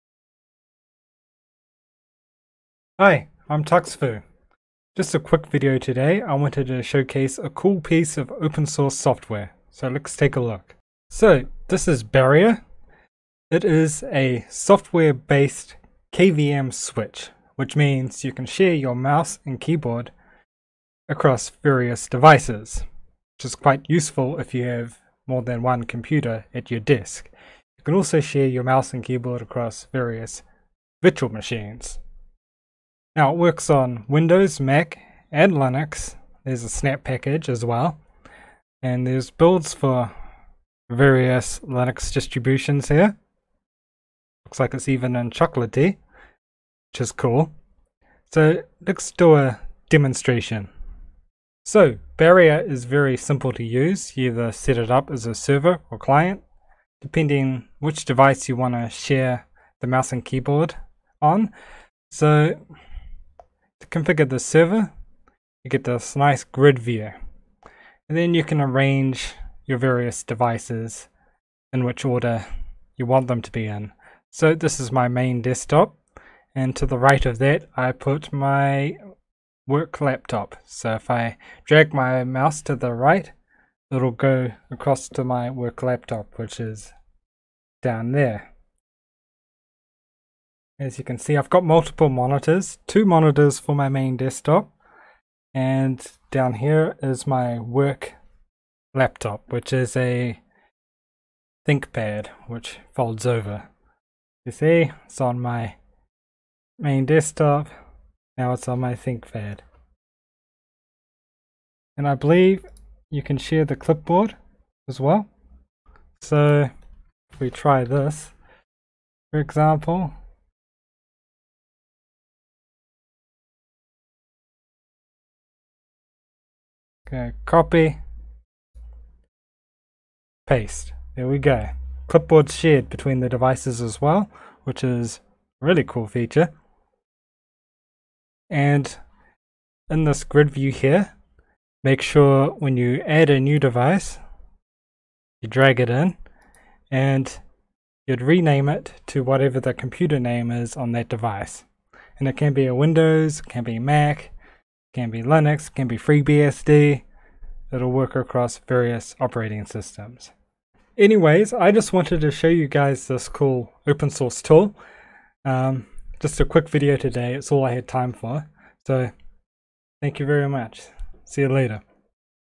Barrier - Open-Source Software KVM Switch
Just showing off a cool open-source tool that allows you to share your mouse and keyboard across computers and VMs

This video will also be on lbry(decentralized alternative to youtube)
For the best experience on lbry, I recommend using the client.